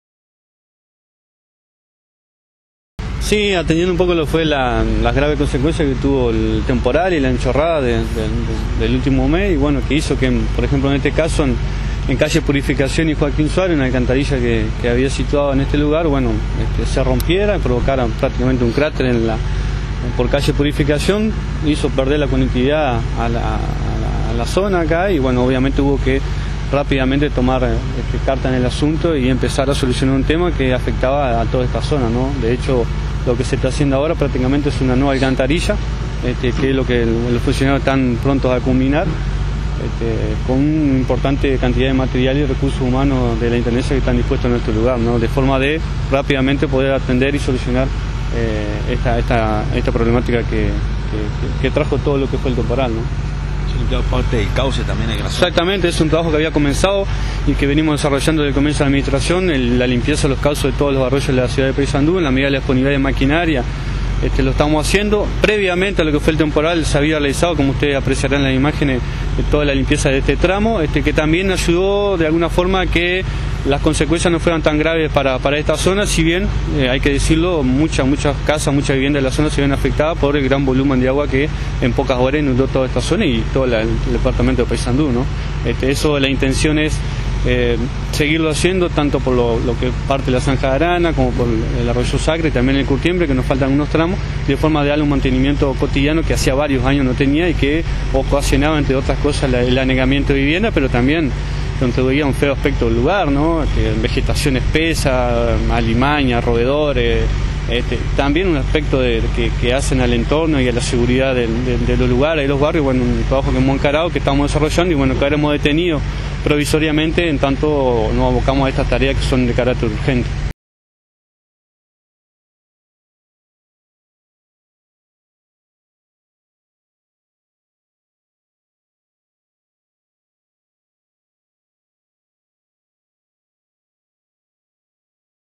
Rápida respuesta a los vecinos
Culminan alcantarilla en Purificación y Joaquín Suárez